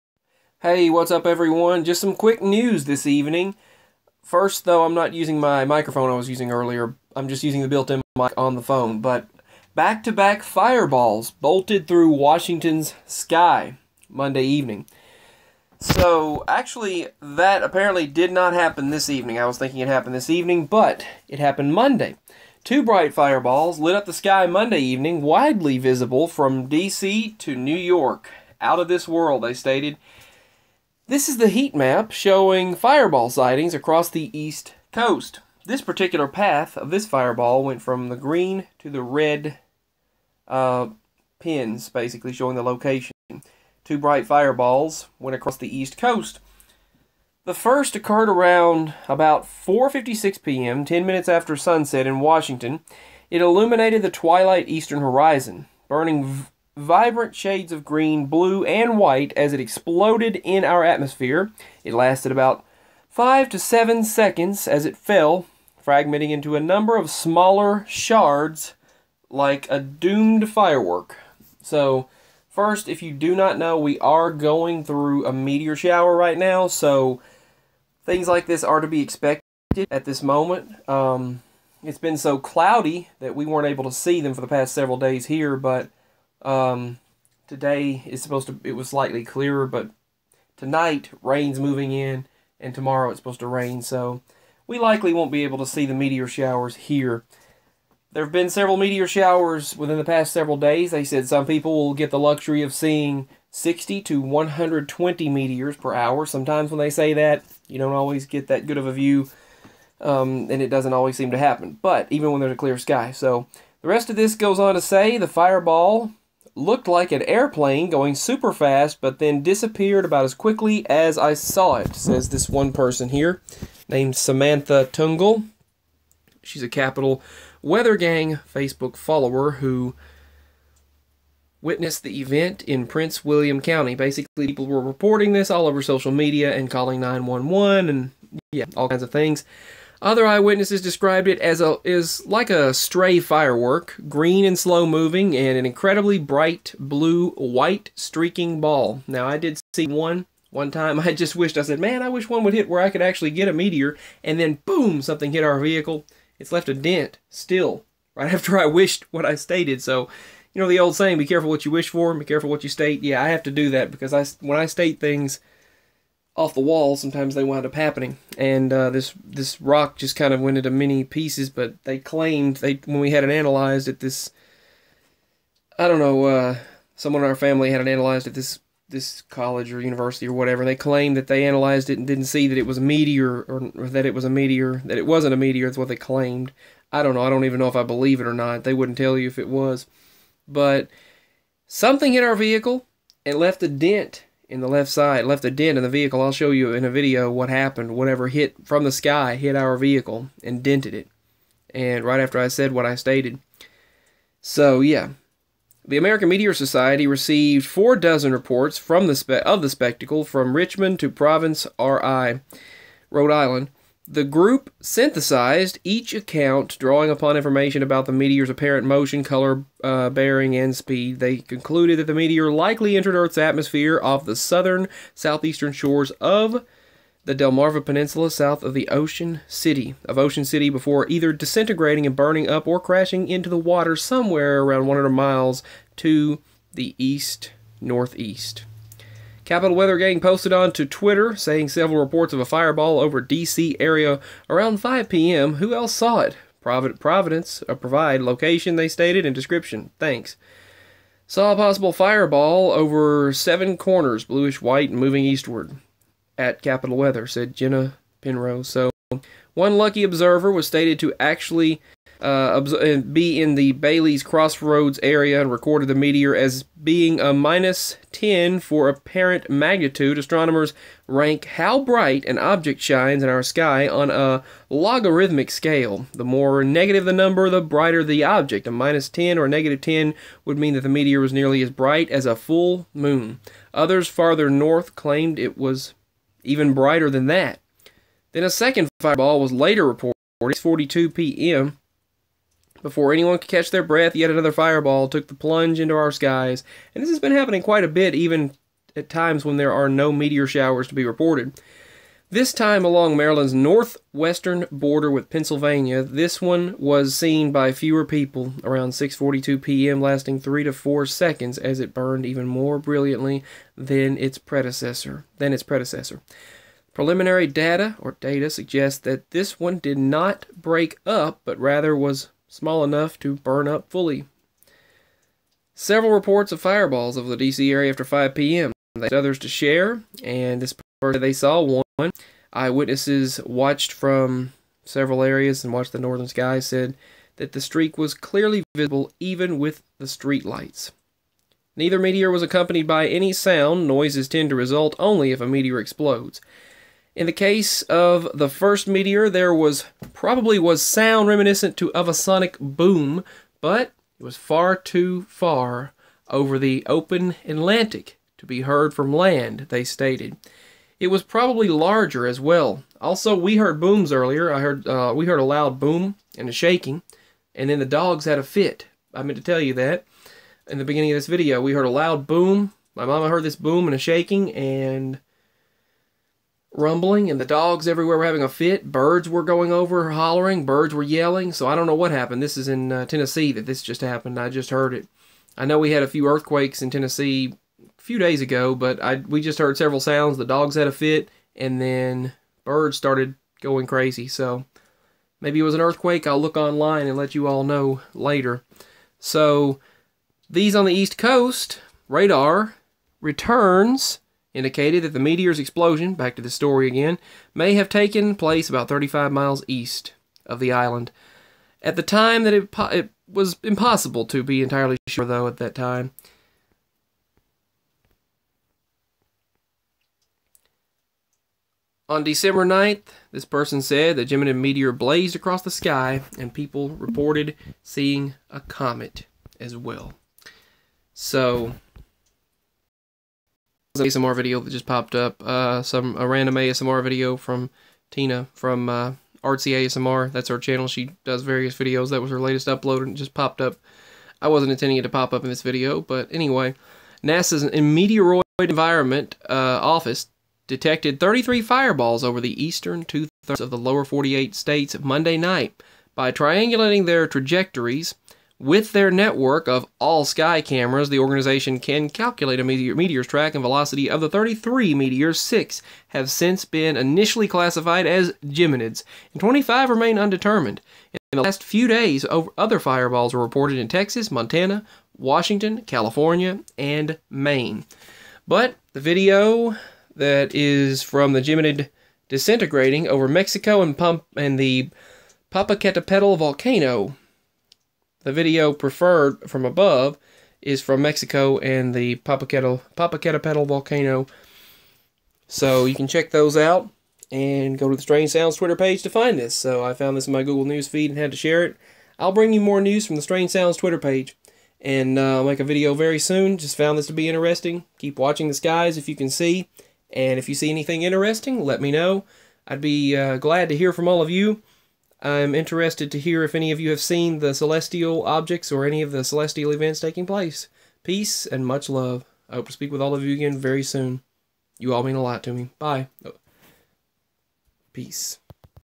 Bright Fireballs Lighting Up The Skies! (Did You See Any of Them?)
These are the latest reports of the meteors from the meteor shower.